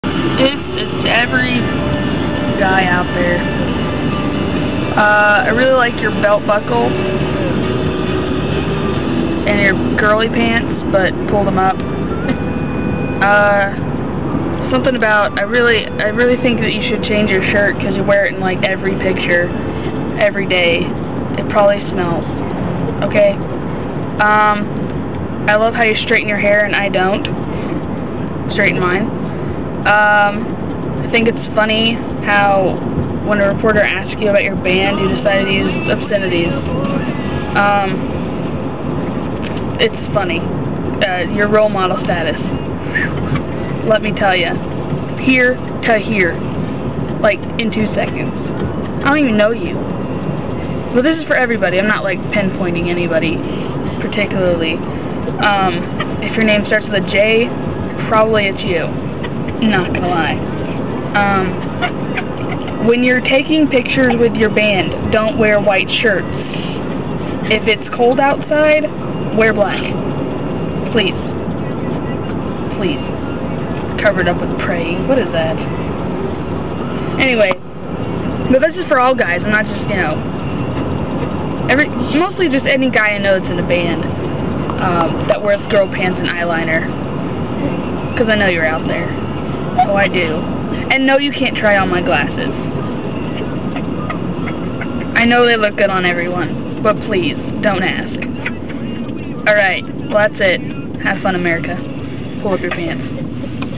Boys...
Heather Giving all guys a lecture...especially if your name starts with a J!